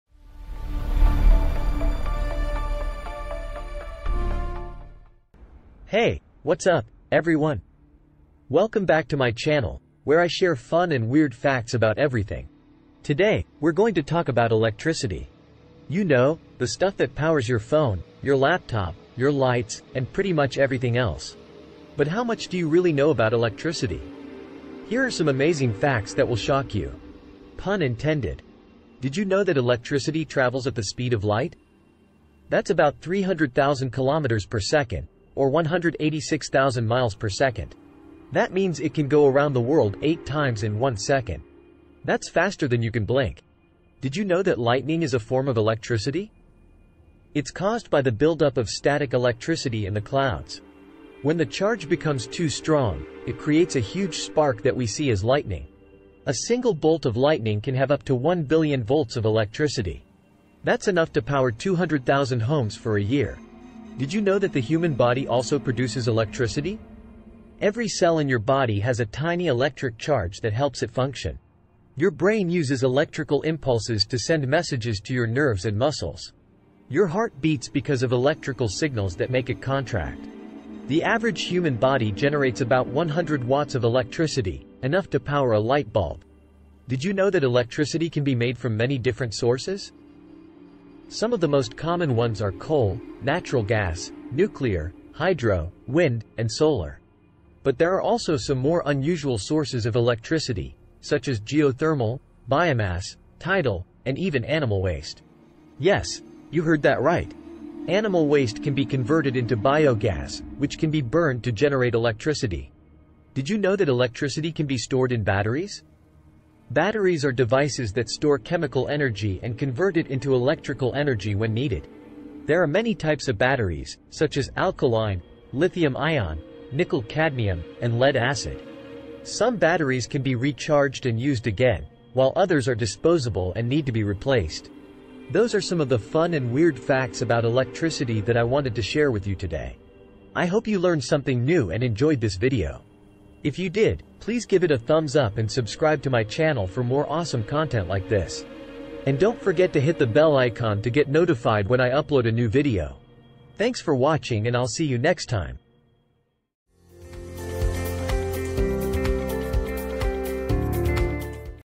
Weird and Fun Facts About Electricity!
In this video, I share some fun and weird facts about electricity that will shock you. You'll learn how fast electricity travels, how powerful lightning is, how your body produces electricity, and more. Watch this video to discover some amazing things about electricity that you probably didn't know.